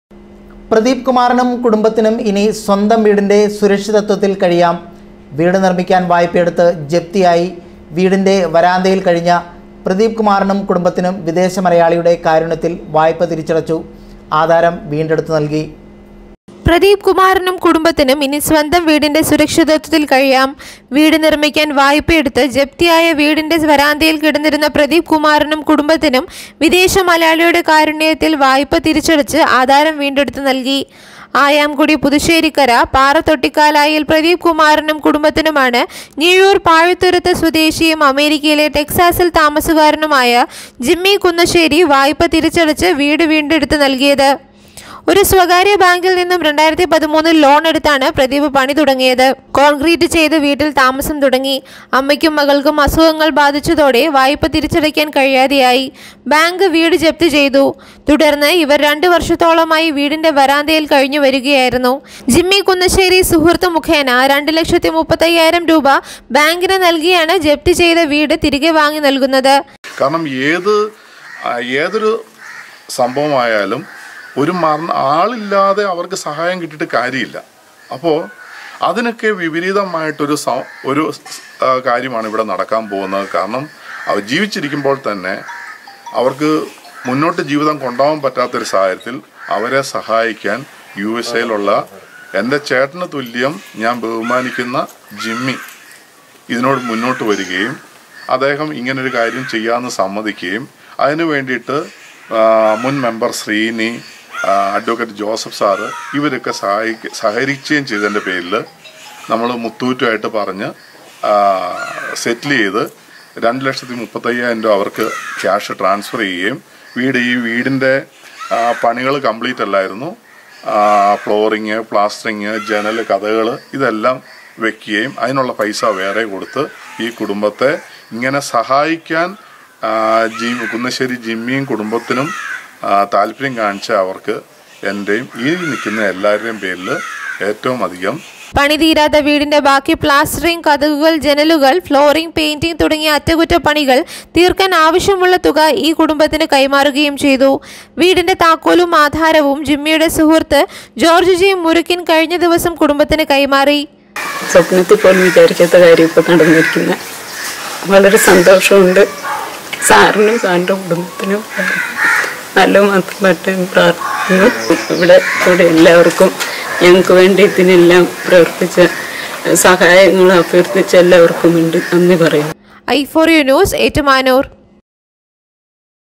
ഇനി സ്വന്തം വീടിന്റെ സുരക്ഷിതത്വത്തില്‍ കഴിയാം
പ്രദീപ് കുമാറിനും കുടുംബത്തിനും ഇനി സ്വന്തം വീടിന്റെ സുരക്ഷിതത്വത്തില്‍ കഴിയാം. വീട് നിര്‍മ്മിക്കാന്‍ വായ്പയെടുത്ത് ജപ്തിയായി വീടിന്റെ വരാന്തയില്‍ പ്രദീപ് കുമാറിനും കുടുംബത്തിനും വിദേശ മലയാളികയുടെ കാരുണ്യത്തില്‍ വായ്പ തിരിച്ചടച്ചു ആധാരം വീണ്ടെടുത്ത് നല്‍കി.